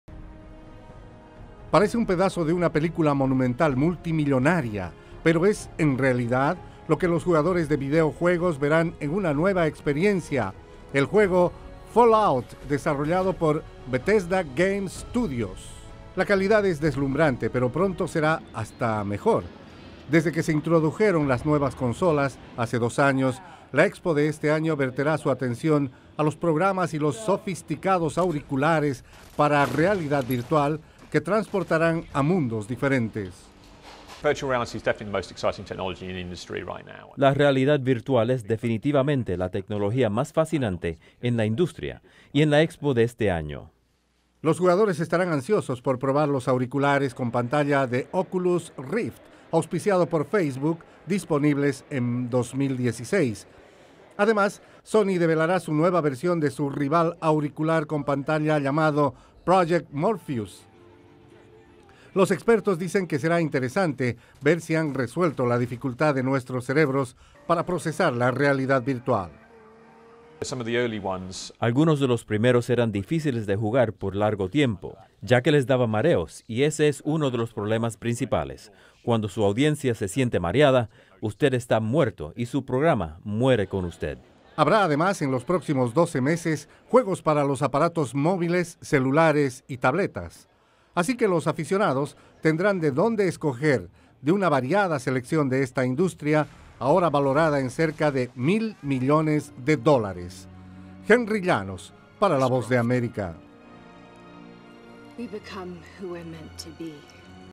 Expo E3 realidad virtual
Comenzando este martes, casi 300 exhibidores mostrarán sus aparatos electrónicos y programas de realidad virtual, que los consumidores están ansiosos por probar, ya que prometen llevar a los videojuegos a un nuevo nivel de tecnología.